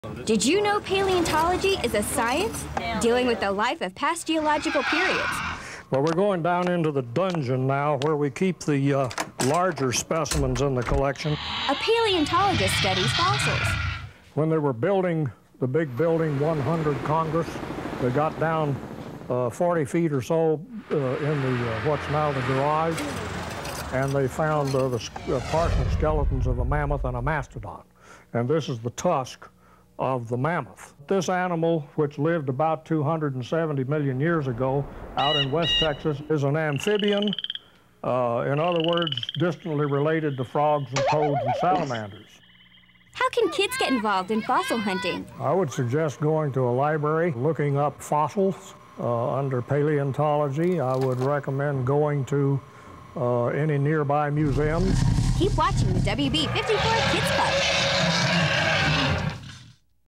Kids Club cool career vignette - paleontologist
An educational commercial about paleontology and dinosaurs found in Austin, Texas.